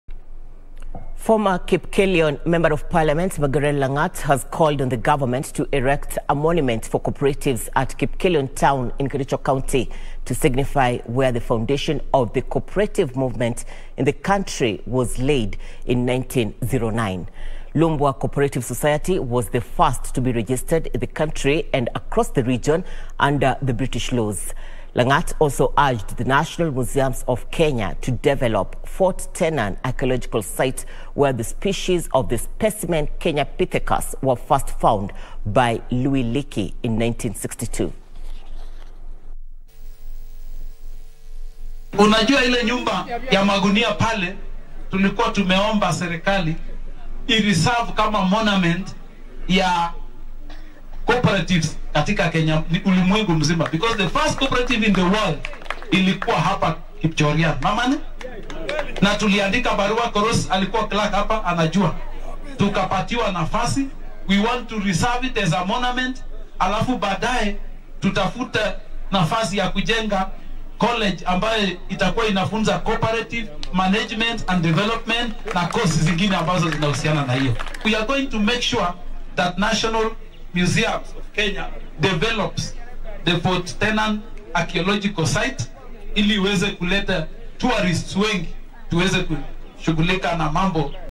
Lumbwa Cooperative society was the first to be registered in the country
Former Kipkelion MP Magerer Langat has called on the government to erect a monument for cooperatives at Kipkelion town in Kericho county to signify where the foundation of the cooperative movement in the country was laid in 1909. Lumbwa Cooperative society was the first to be registered in the country and across the region under the British laws. Langat also urged the National Museums of Kenya